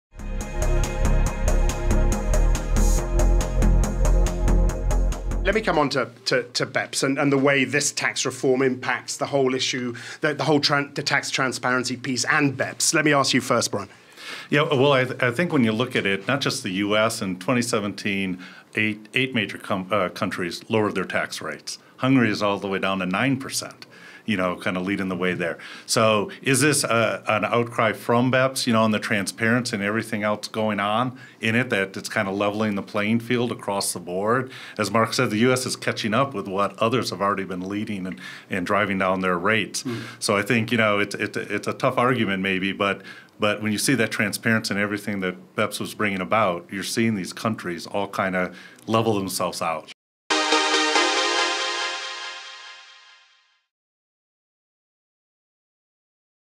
How will tax reform impact tax transparency and BEPS?
Mark Weinberger, EY's CEO, joined Reuters Axel Threlfall and Brian Peccarelli, President of Tax and Accounting, to discuss the 2018 outlook for global tax changes, and the impacts and ramifications of US tax reform.